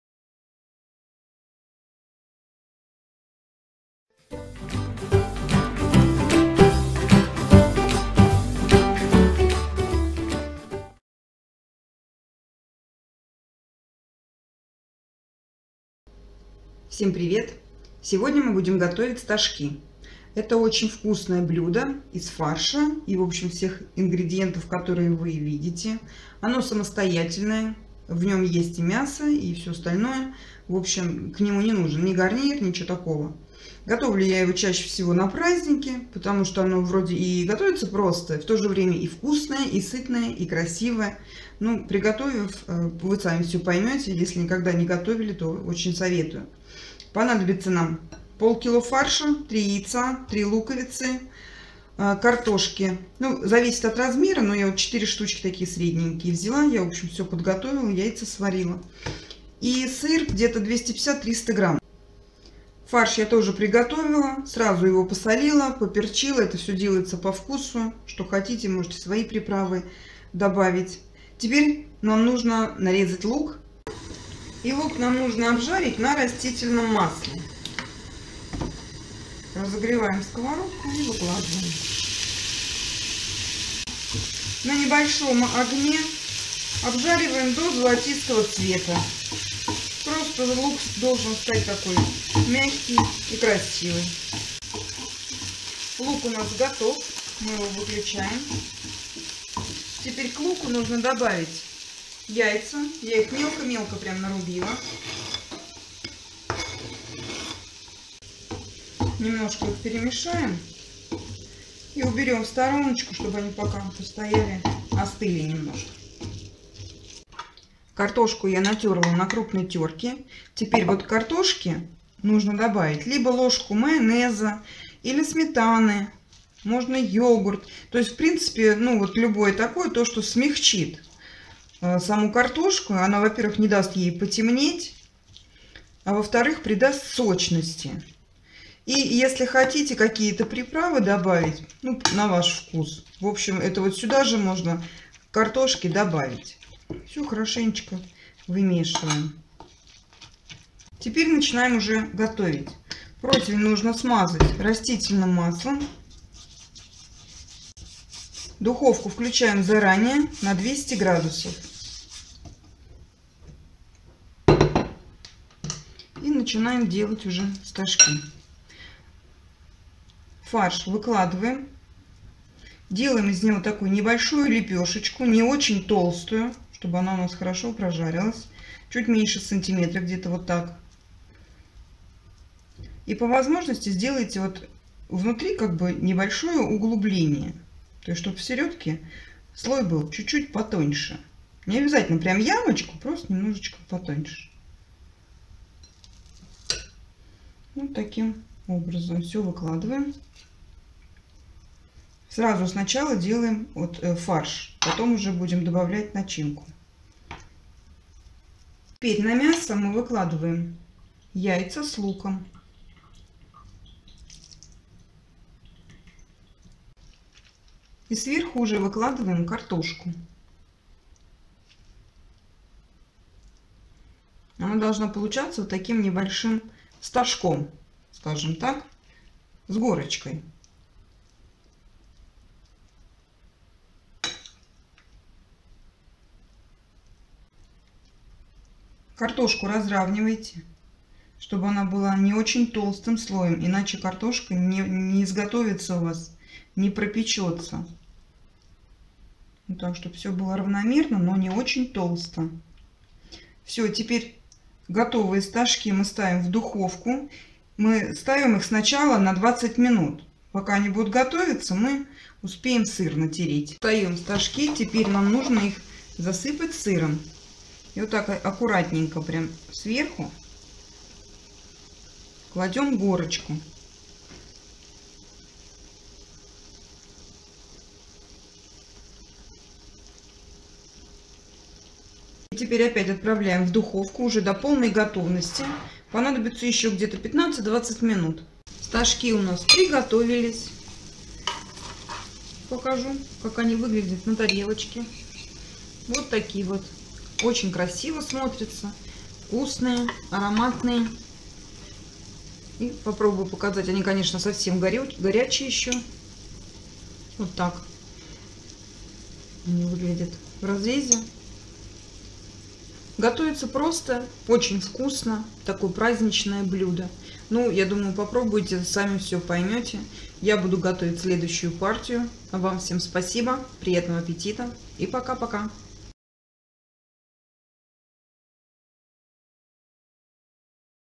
Мясные Стожки из фарша в духовке Очень Вкусные и Сочные! Рецепты на Новый Год.
Рецепт. Стожки из мясного фарша видео. Стожки из фарша с сыром. Мясные стожки приготовление. Стожки из мяса.
Ингредиенты: фарш 0,5 кг, 3 луковицы, 3 яйца, картофель 4 шт, сыр 250-300 гр, майонез или сметана 1 ст. ложка, соль и приправы по вкусу.                                                                                          mince 0.5 kg, 3 onions, 3 eggs, potatoes 4 pieces 250-300 g of cheese, mayonnaise or sour cream 1 tbsp. spoon, salt and seasoning to taste in oven for 20 mins, add cheese and another 15-20 mins ( 200 degrees).